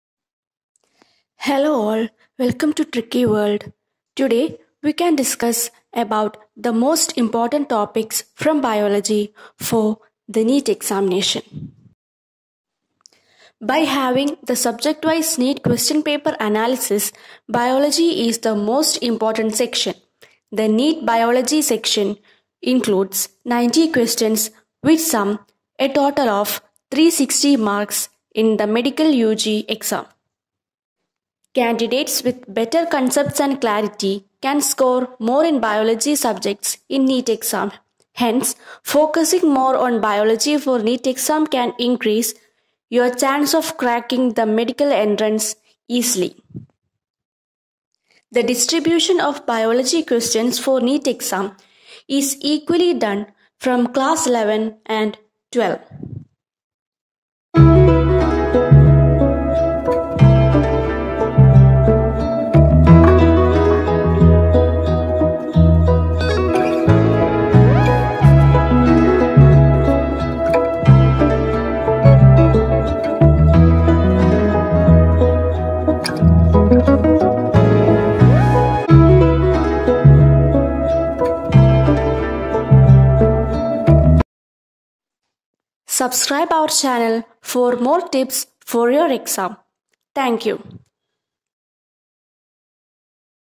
NEET toppers do or die topics for neet || Final call neet rankers biology strategy
neet | neet 2021 | neet 2022 | neet toppers | do or die topics for neet |

Tricky world brings to you one shot session on DO OR DIE TOPICS FOR NEET BIOLOGY